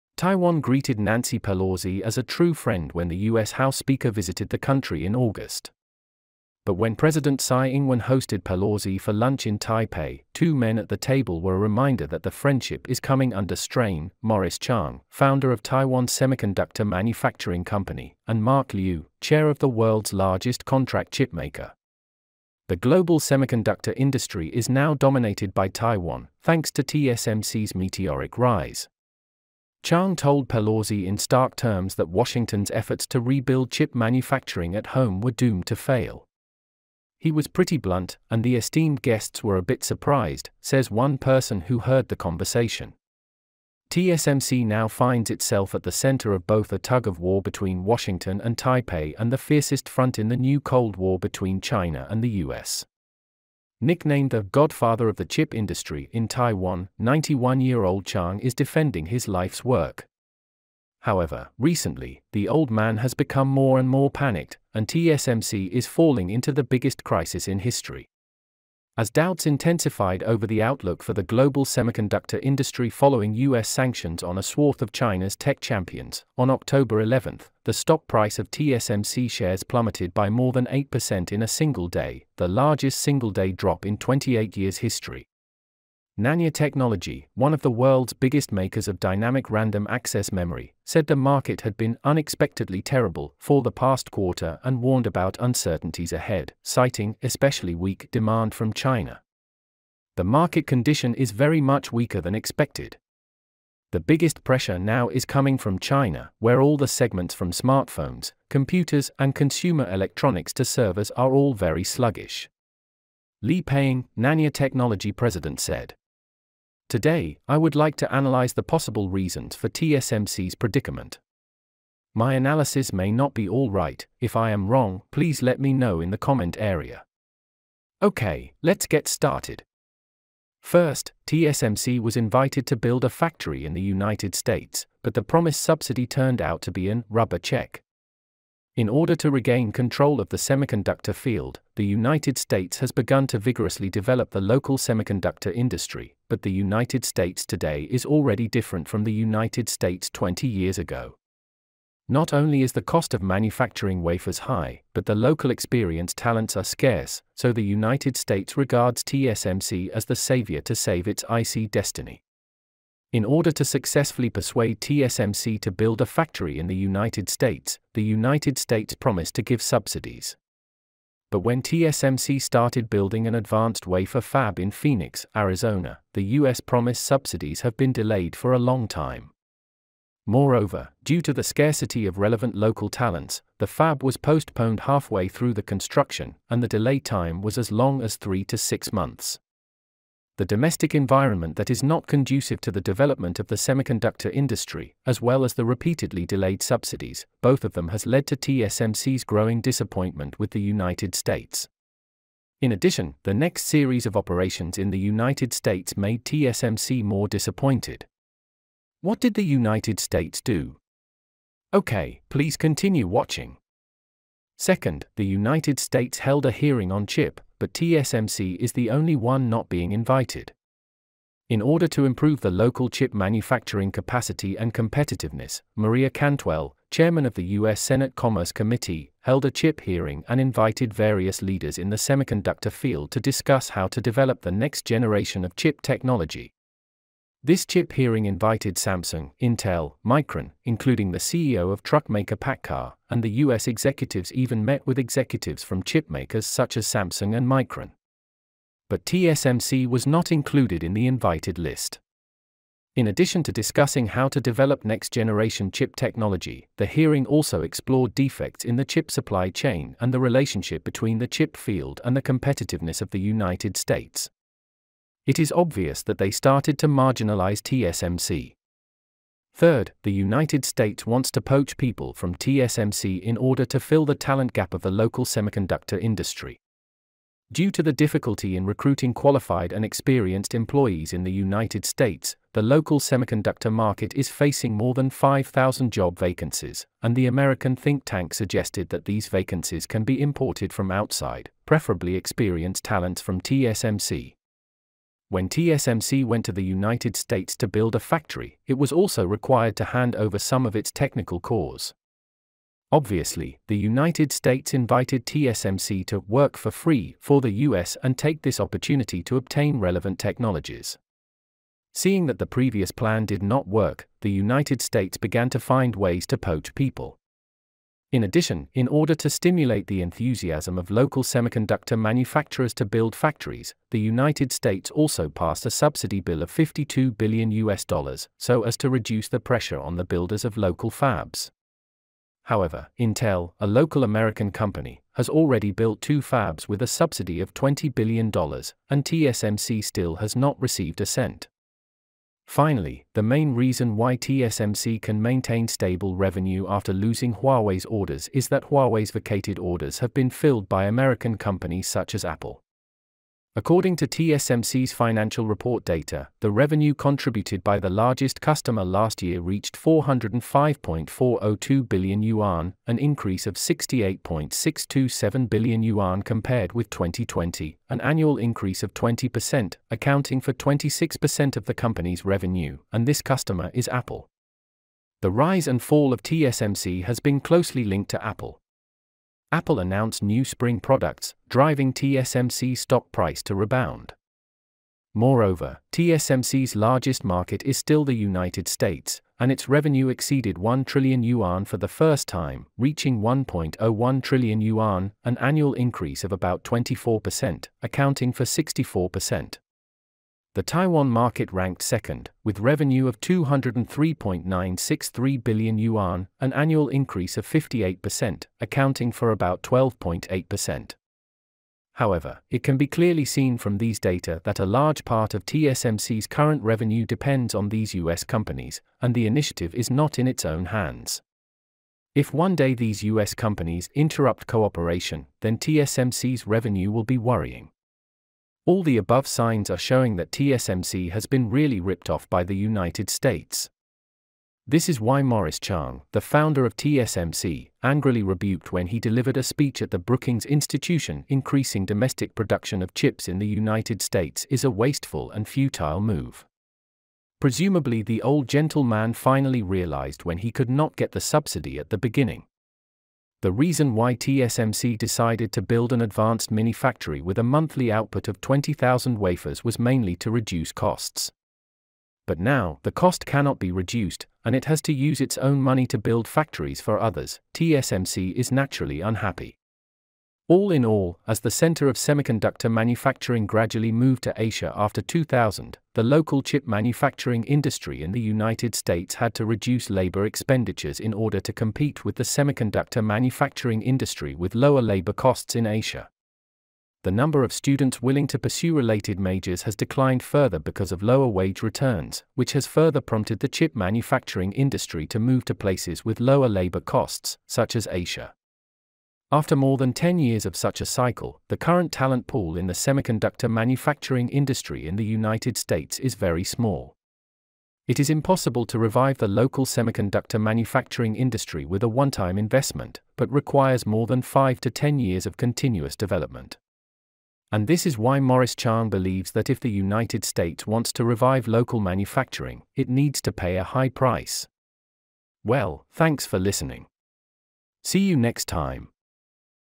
Who should be blamed for the biggest crisis in the history of TSMC?
TSMC is falling into the biggest crisis in history. As doubts intensified over the outlook for the global semiconductor industry following U.S. sanctions on a swath of China's tech champions, on October 11, the stock price of TSMC shares plummeted by more than 8% in a single day, the largest single-day drop in 28 years history.
Nanya Technology, one of the world's biggest makers of dynamic random access memory, said the market had been "unexpectedly terrible" for the past quarter and warned about uncertainties ahead, citing "especially weak" demand from China. "The market condition is very much weaker than expected.
The biggest pressure now is coming from China, where all the segments from smartphones, computers and consumer electronics to servers are all very sluggish." Lee Pei-ing, Nanya Technology president said. Today, I would like to analyze the possible reasons for TSMC's predicament.
OK, let’s get started!